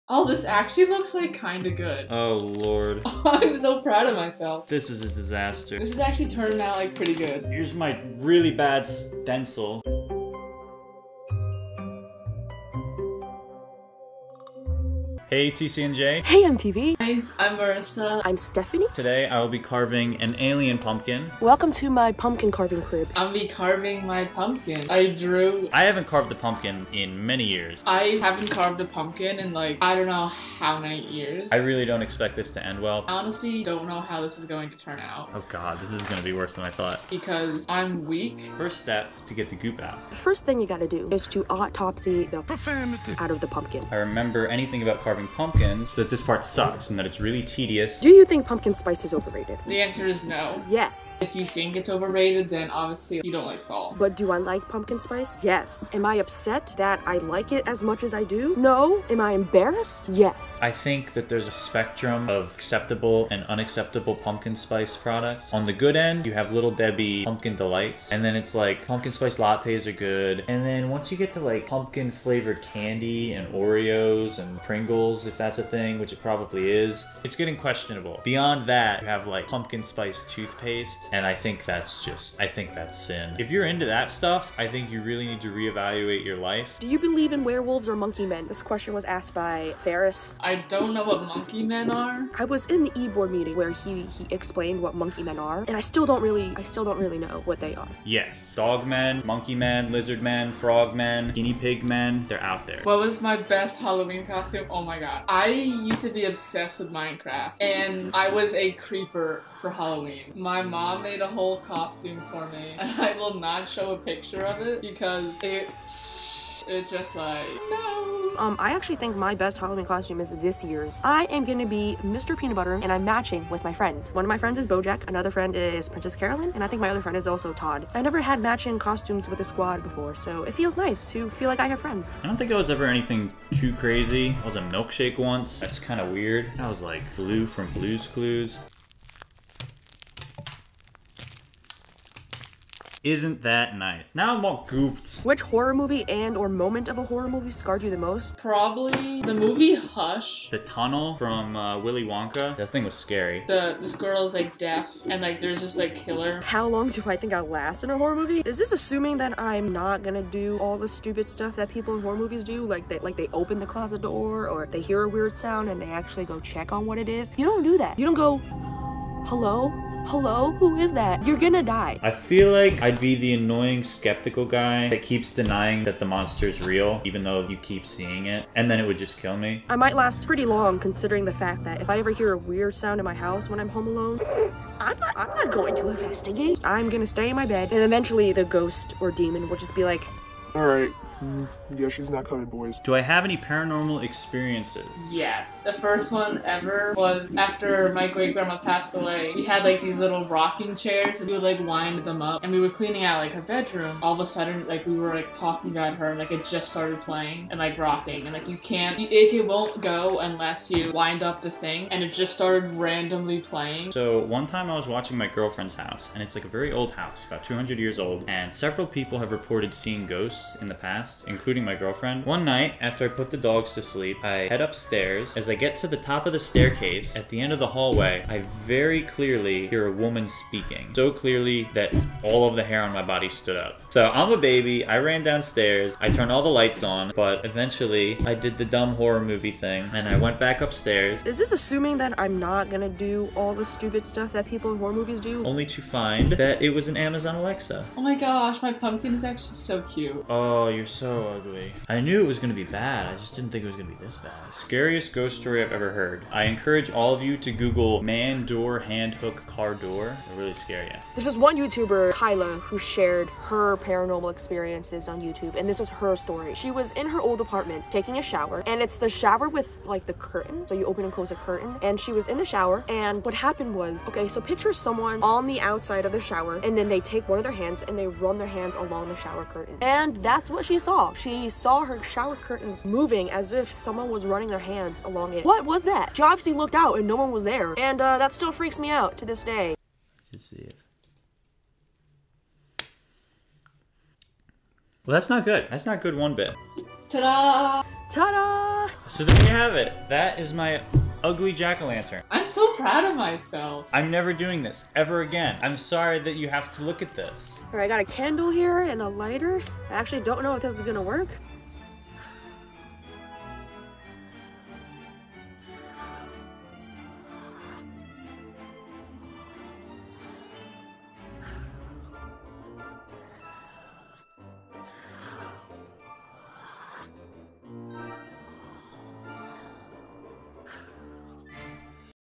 Pumpkin Carving & Spooky Q&A 🎃 👻 | Life@TCNJ
In this month's episode of Life at TCNJ, Join Bryan ('24), Marissa ('24), and Stephanie ('22) for a pumpkin carving & spooky Q&A session to get y'all in the mood for Halloween 🎃

Edited by: Stephanie Shen (Executive Producer of Life @ TCNJ)

TCNJ Lions Television is the official television station of TCNJ.